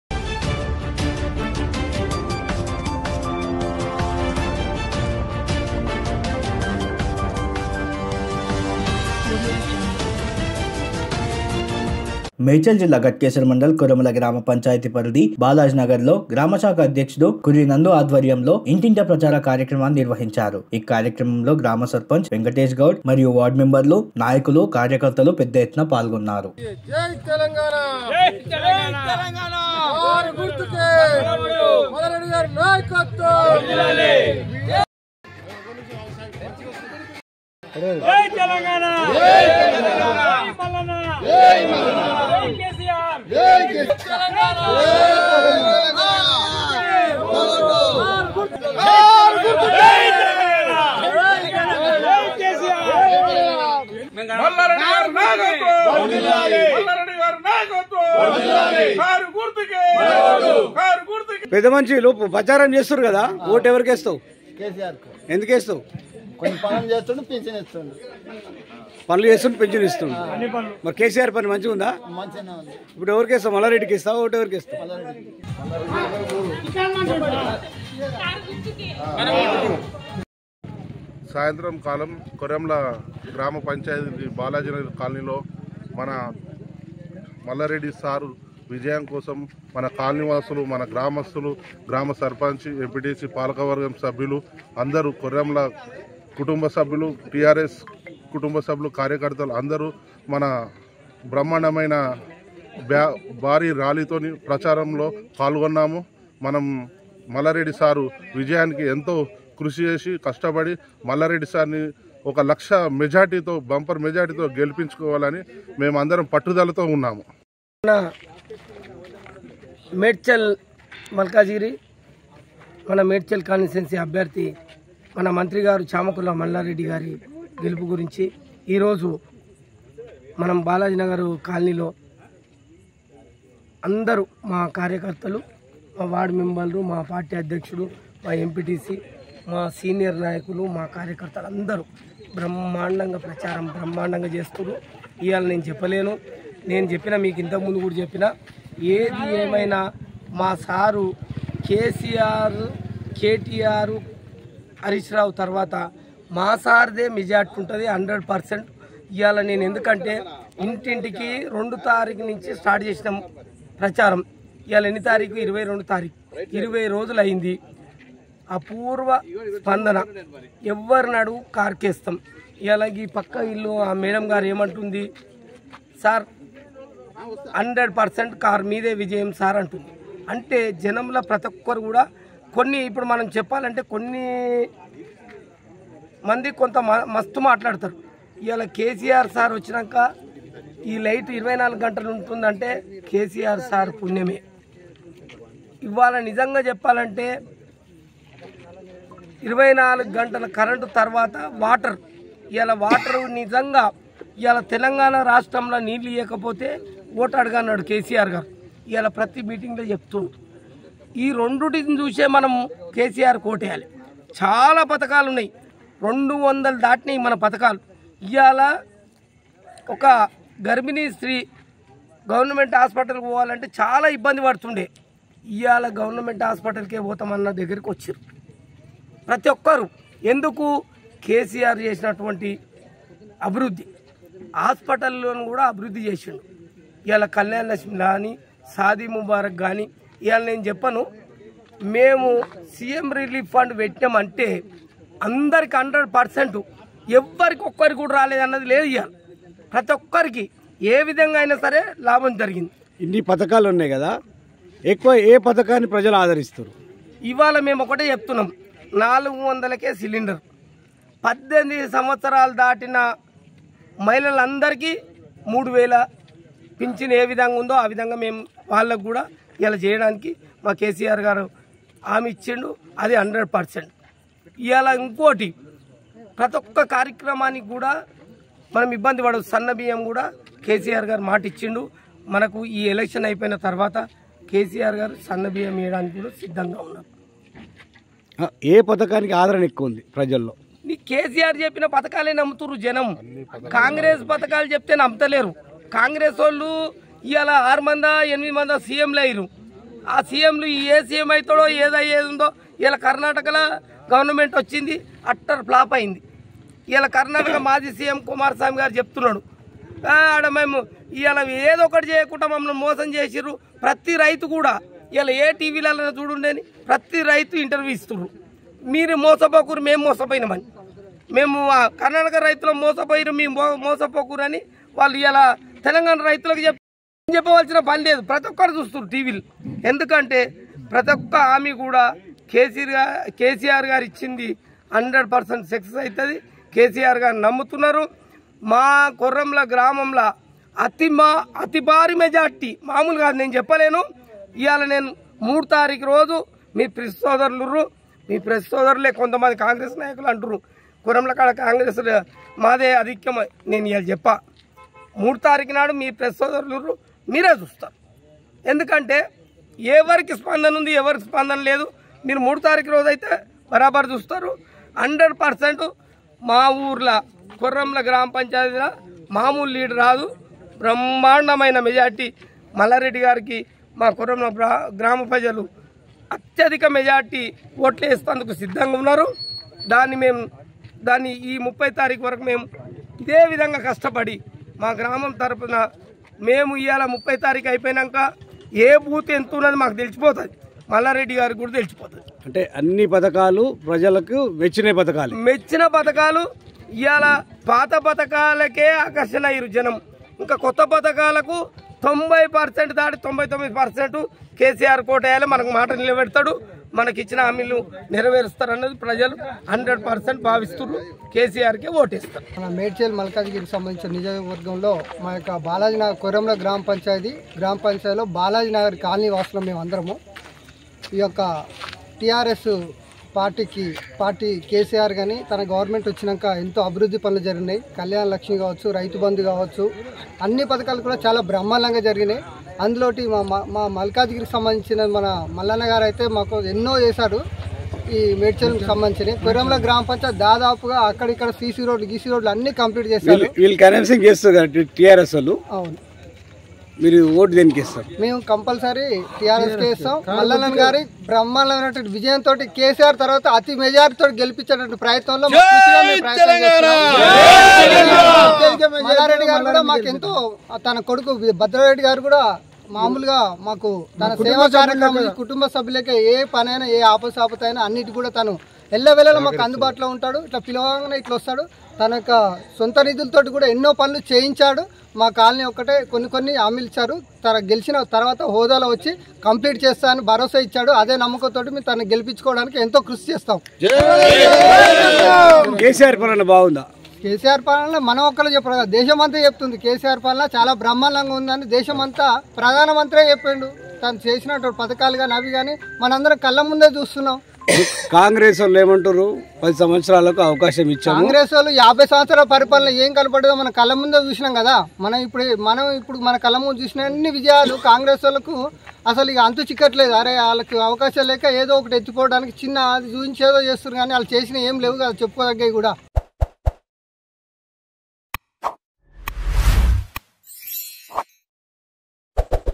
బాలాజీ నగర్ లో జోరుగా బీఆర్ఎస్ పార్టీ ప్రచారం || D9 TV TELUGU
D9 TV TELUGU | DMC TELUGU